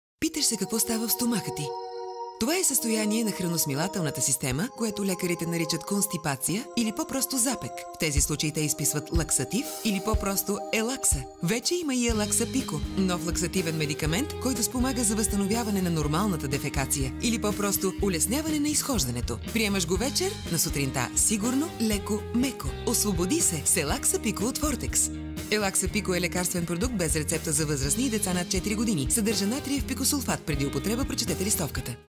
✅Елакса Пико - освободи се!✅
ЕЛАКСА пико е локално действащо слабително средство, което стимулира чревната перисталтика и омекотява изпражненията.
Като слабително средство, което проявява терапевтичното си действие върху дебелото черво, ЕЛАКСА пико стимулира по специфичен начин естествения процес на изпразване на долната част на гастроинтестиналния тракт. Затова ЕЛАКСА пико не оказва въздействие върху храносмилането, както и не води до натрупване в тънкото черво на калории или други хранителни вещества.
📞 0875 / 322 000 за поръчка
За повече информация потърсете и във физическите аптеки Фрамар и попитайте фармацевтите!